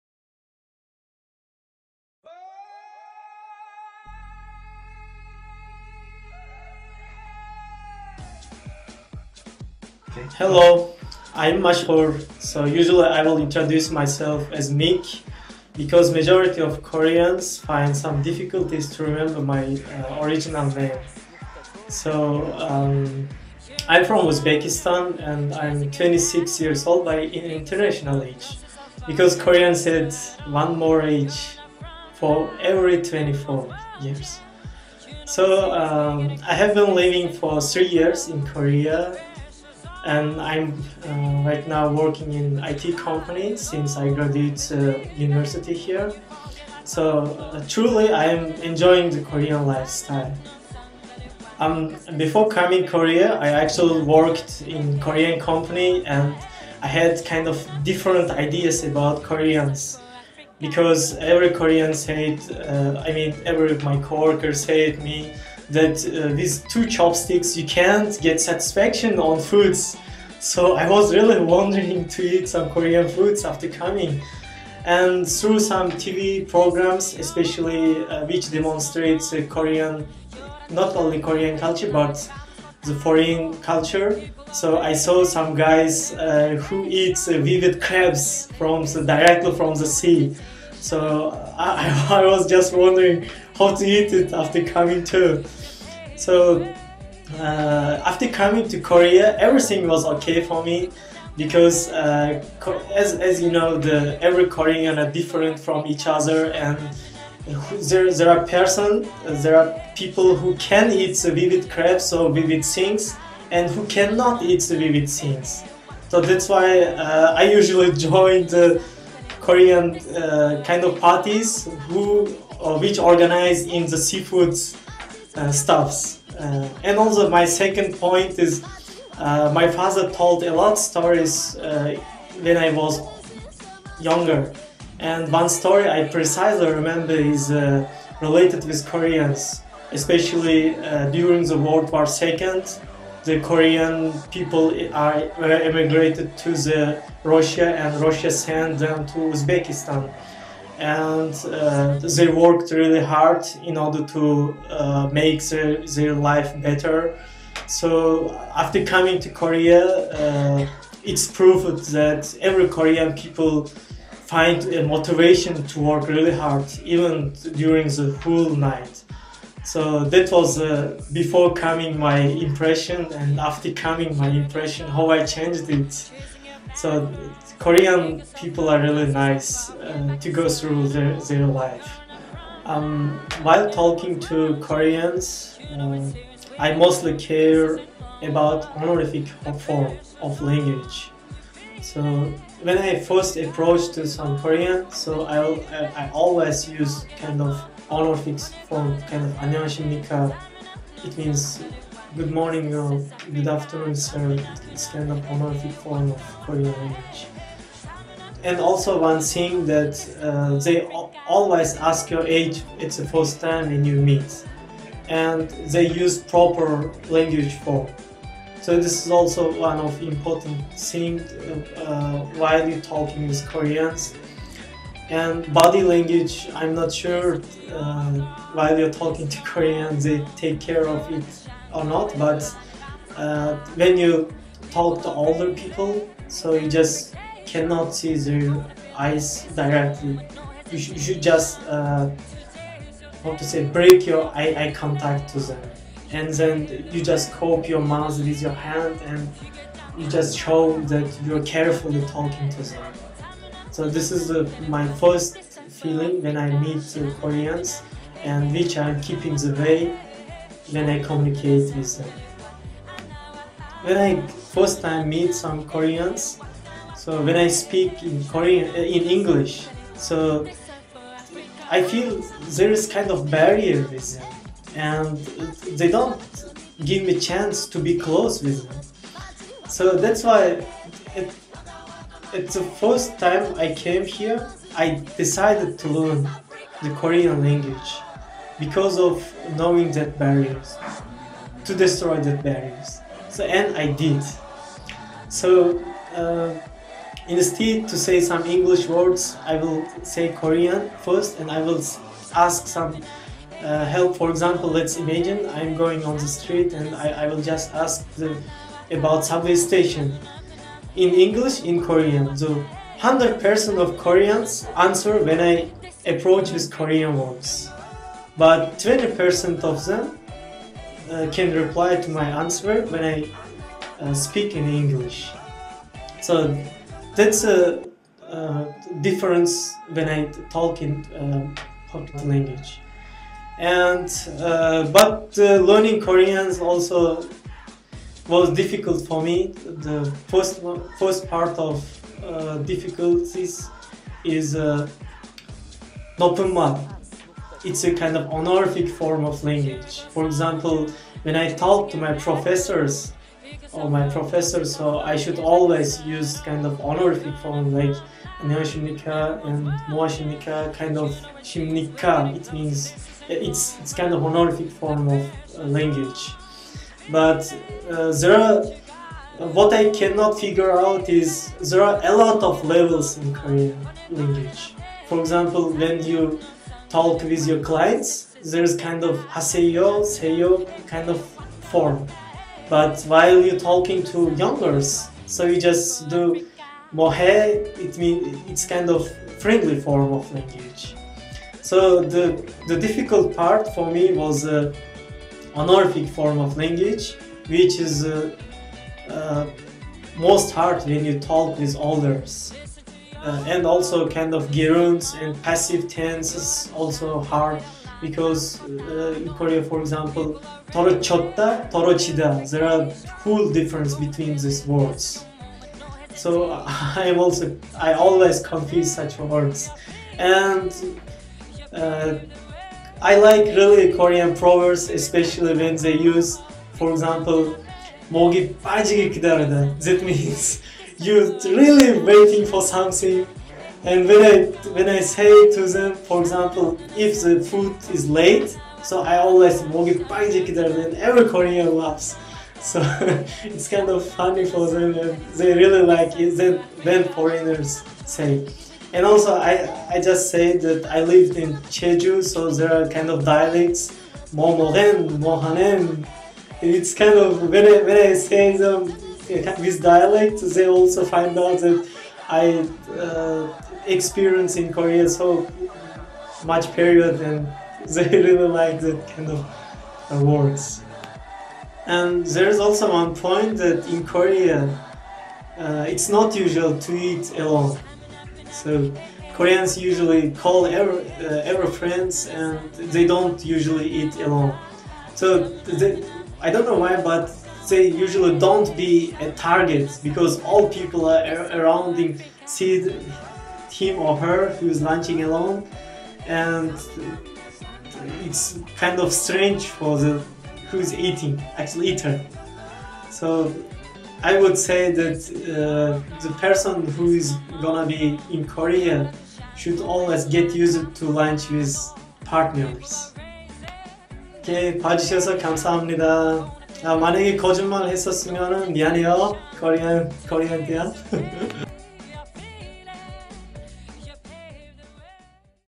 BEANIEHEAD EP4S-Uzbek Perspective
BEANIEHEAD PROJECT Episode 4 Short version - Uzbek Perspective

In the Ep4, Mr. Mashhur Sattorov from Uzbekistan talked about Socializing, Dining culture in Korea and Having difficulty in Learning Korean language.  The video was taken at Gangnam Toz, May 23rd, 2012.

If you have any opinion on the video or Korean culture, please share with us.

BEANIEHEAD PROJECT